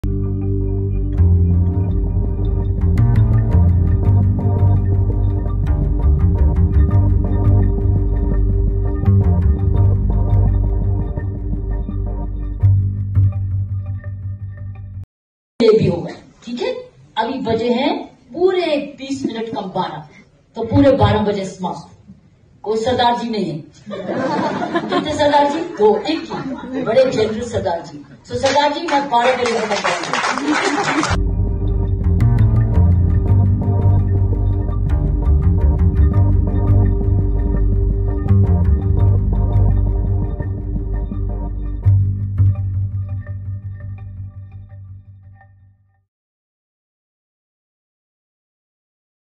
Kiran Bedi's 'Joke' On Sikhs Causes Controversy, AAP Condemns Remarks | Mirror Now
The Aam Aadmi Party hit out at BJP leader Kiran Bedi for making certain remarks against Sikhs during a book launch event, accusing the former Puducherry L-G of hurting religious sentiments. Here is the full story.